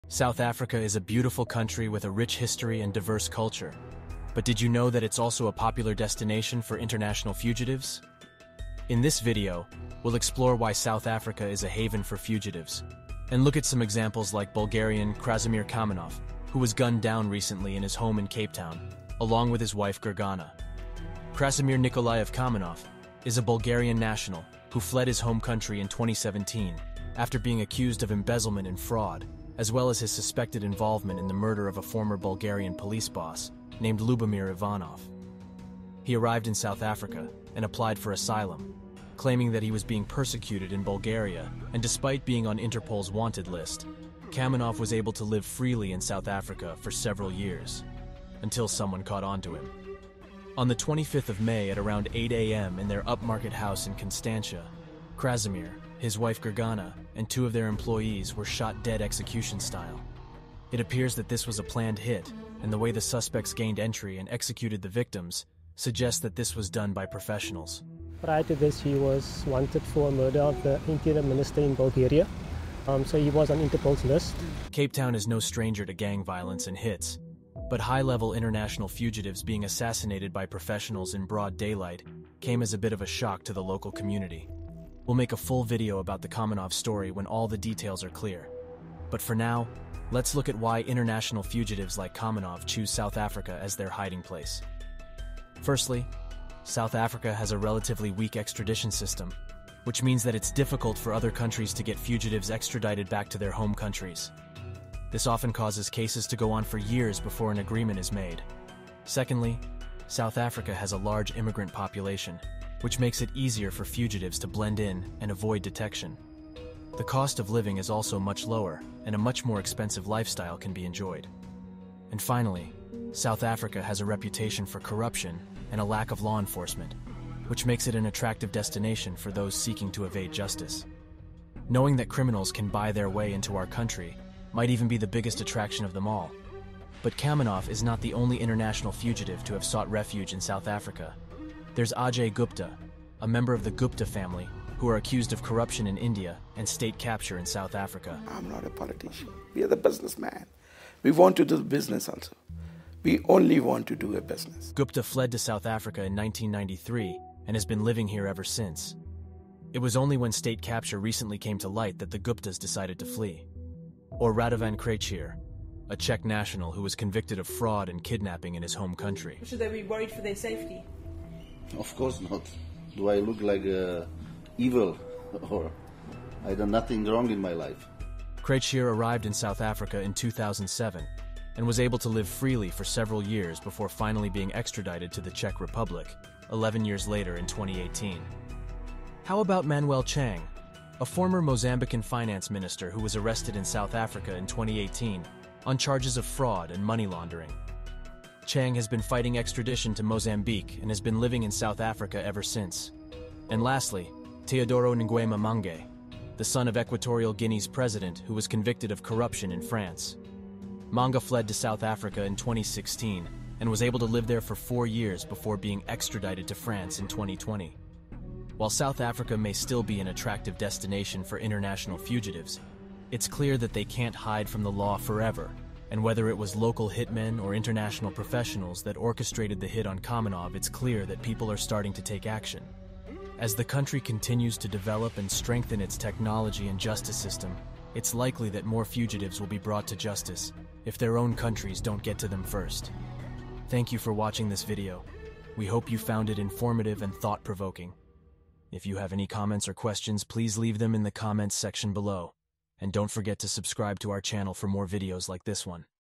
Constantia Gangs | The Hitmen From Bulgaria
breakingnews shortdocumentary In this video we explore the story of The Constantia HIT and shed light on South Africa's reputation as a haven for international fugitives. The case involved Krasimir Kamenov, a Bulgarian national who was wanted by Interpol for his alleged involvement in a mafia-style hit in Bulgaria. Kamenov was able to evade justice for years by living in South Africa, where he had established a new life under a false identity.

Through interviews with law enforcement officials, legal experts, and journalists, we delve into the reasons why South Africa has become a popular destination for international fugitives. We examine the country's weak extradition system, its large immigrant population, and its reputation for corruption and lack of law enforcement. We also explore the human cost of fugitives like Kamenov, who are able to evade justice and continue living their lives while their victims and their families are left to suffer.

This video provides a thought-provoking look at the complex issues surrounding international fugitives and their choice to live in South Africa. It raises important questions about the country's justice system and its role in the global fight against crime.